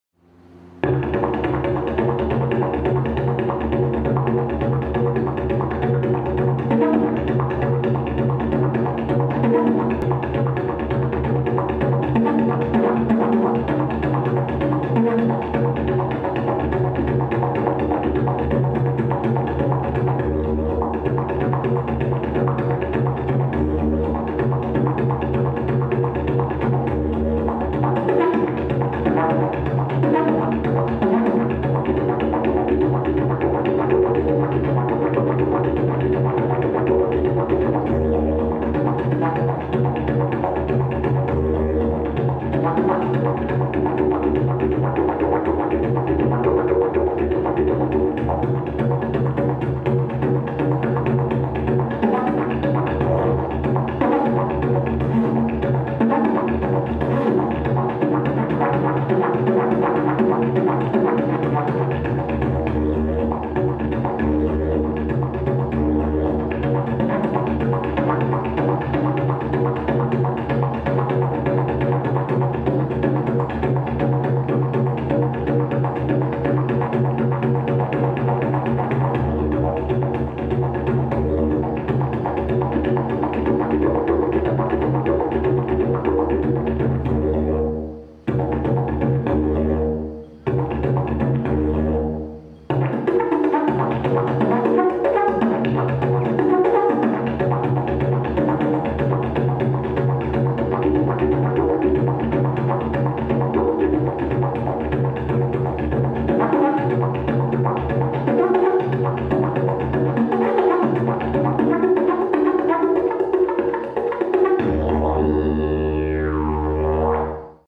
Awayo Didgeridoo
Sound test of my didgeridoo made by Awayo with an Andean fabric from Bolivia

Length: 194cm
Mouthpiece: 30mm
Key E sharp
phosphorescent mouthpiece